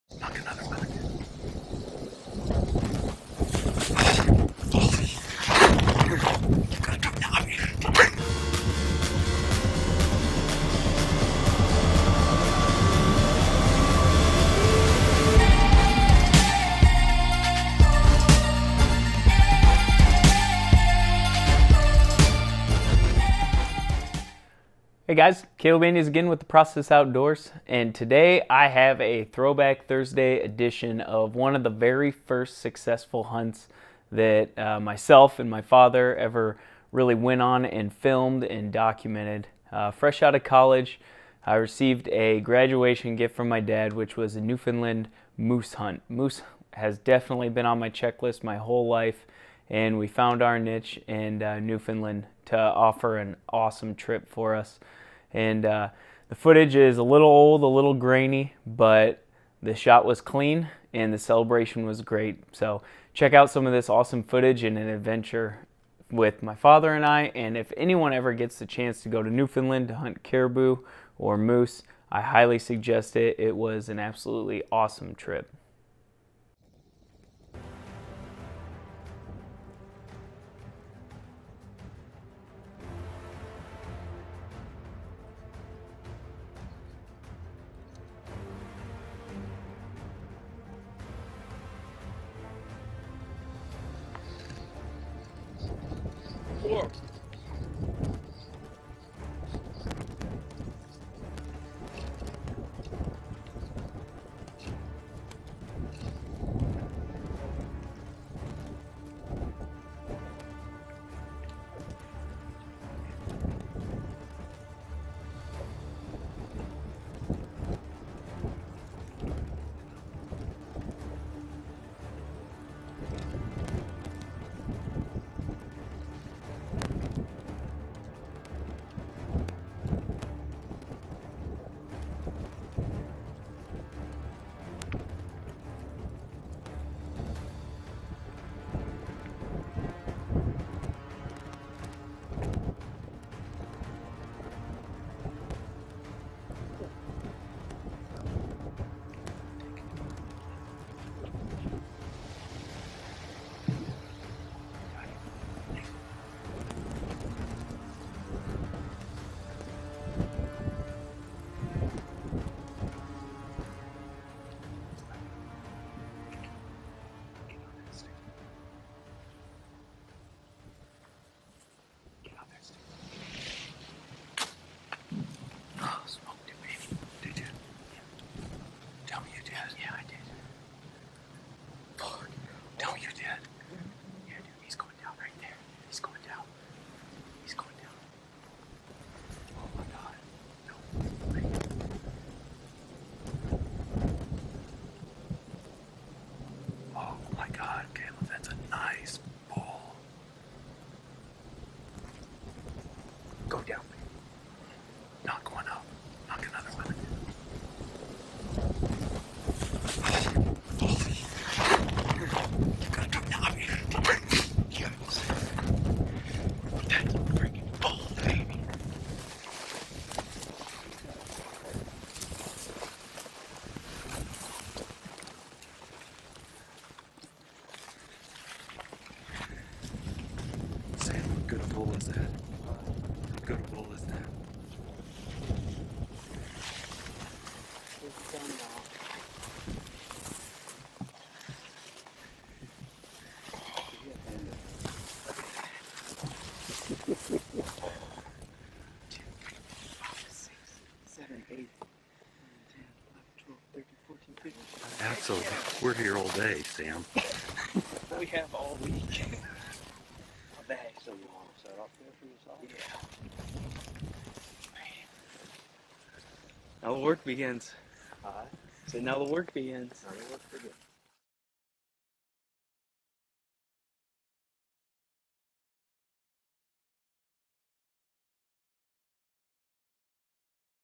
Moose at 7 Yards!
TPO team member Caleb Andes first successfully filmed hunt is on a New Foundland bull of a lifetime.

NFRRSBDPU9IXRHDR